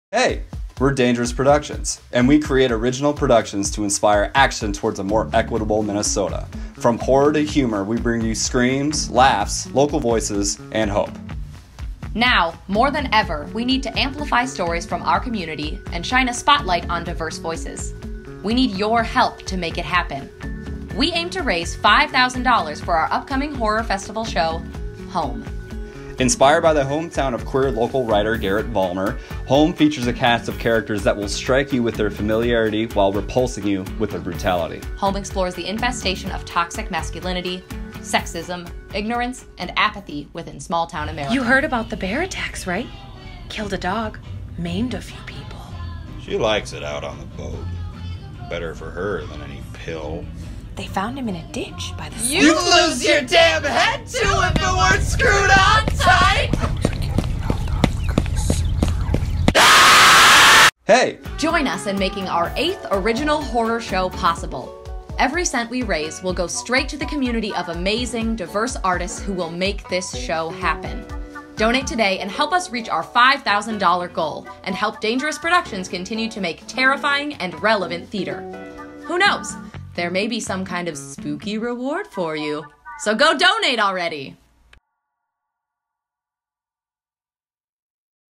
DP Fundraiser HOME
Donate today to make Dangerous Productions' next horror production, HOME by Garrett Vollmer, happen at this year's Twin Cities Horror Festival!